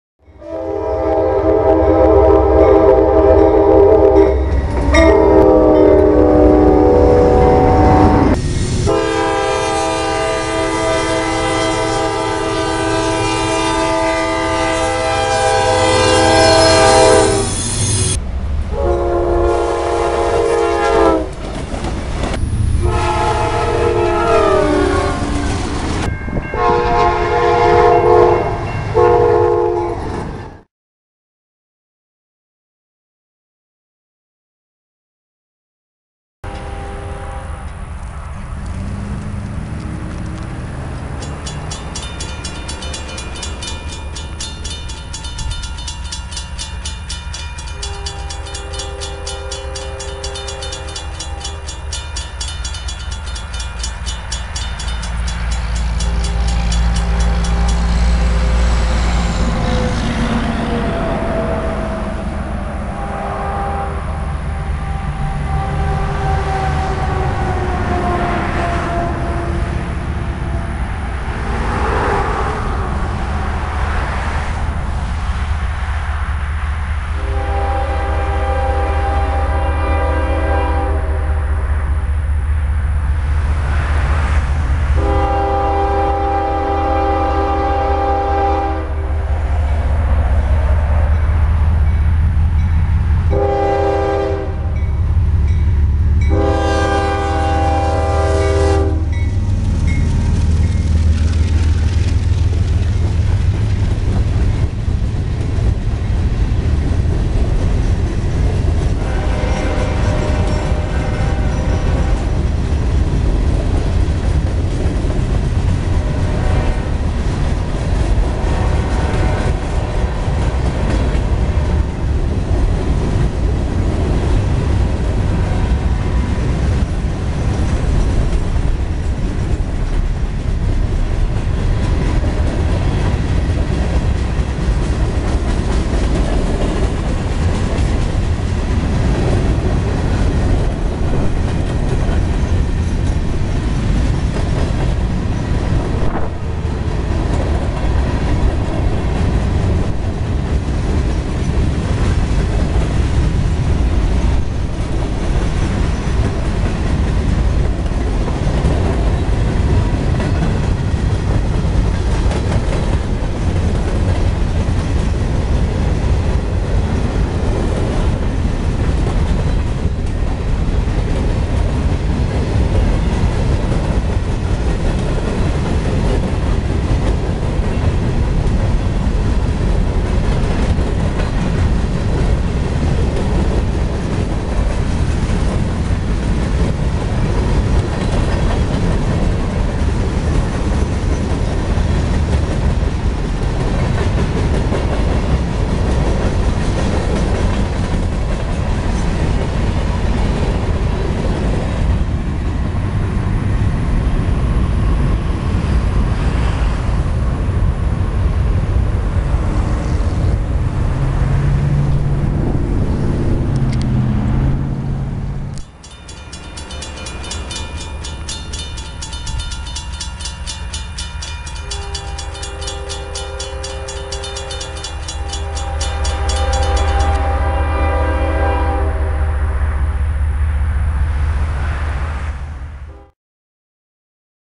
CSX Train Hauling Empty Gondola Cars
This is a video I made of an empty gondola train that went by my home earlier that I raced to a crossing 10 miles ahead to film for you.